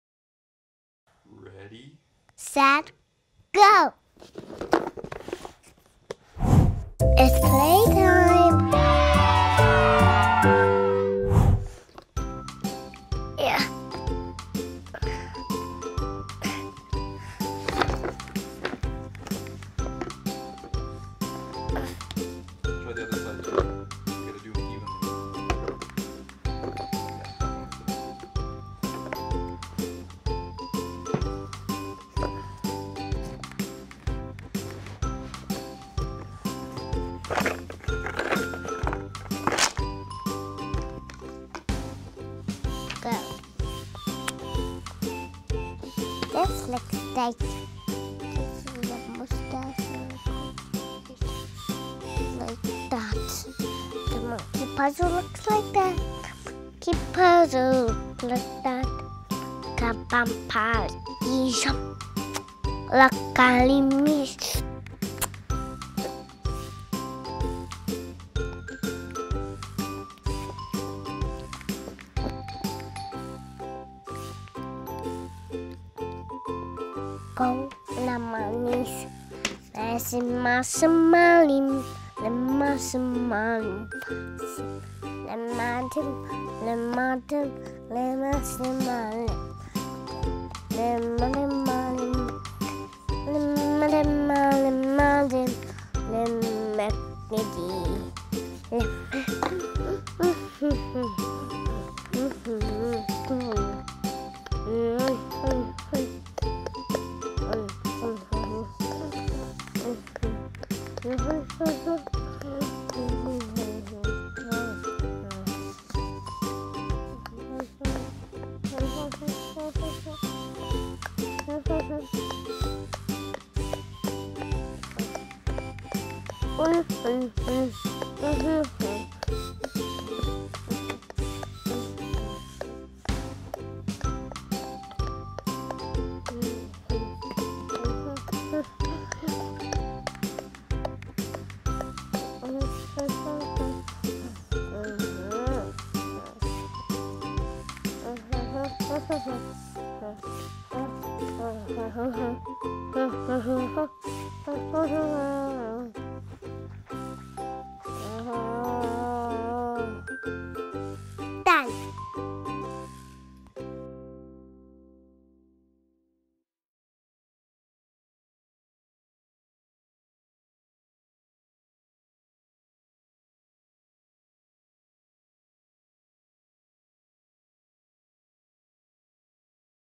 Crocodile Creek Little Monkey Jigsaw Puzzle Review (12-Piece)
An excellent addition to the puzzle collection. This 12-piece jigsaw puzzle is made of fairly high quality cardboard-type material. Although not as thick, sturdy and durable as the Melissa & Doug wooden puzzles, it still holds its own very well against other, similar puzzles made of the thinner, cheaper, cardboard-type material. The box it comes in also doubles as a storage bin for the pieces so it's easier to put away when you're done.